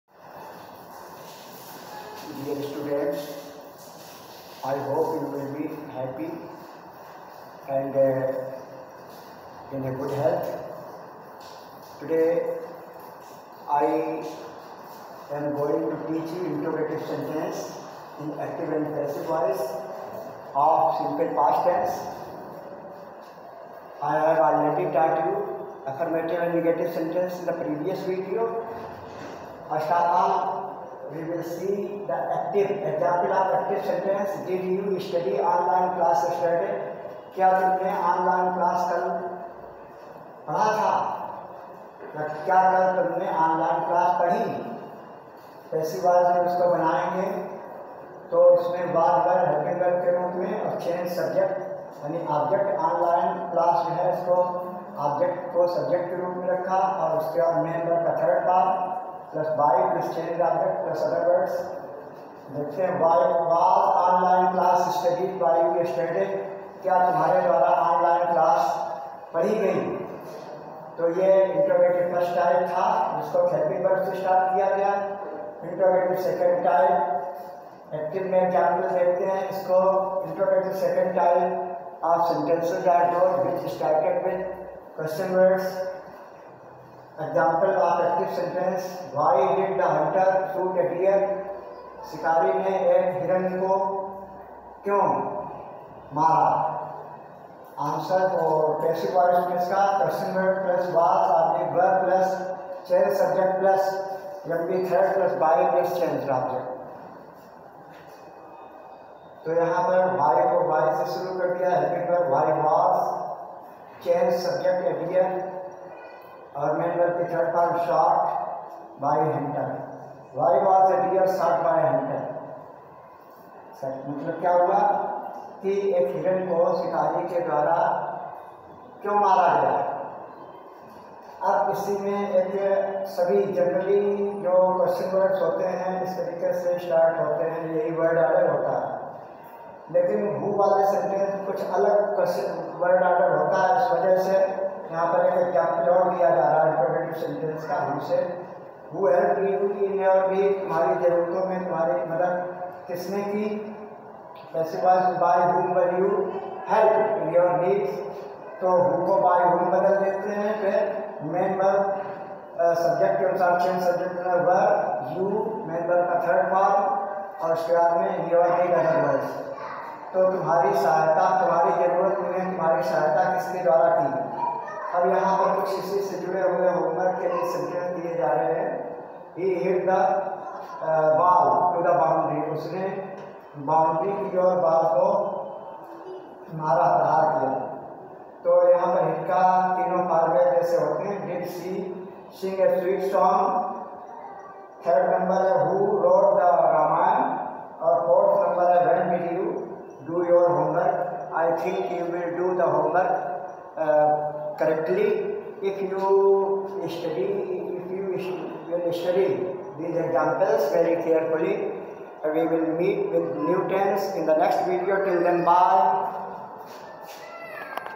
Class : 8 Sub:Eng. Lang.Topic: Voice of Simple Past taught by Subhash Sir
26 Aug.2020Share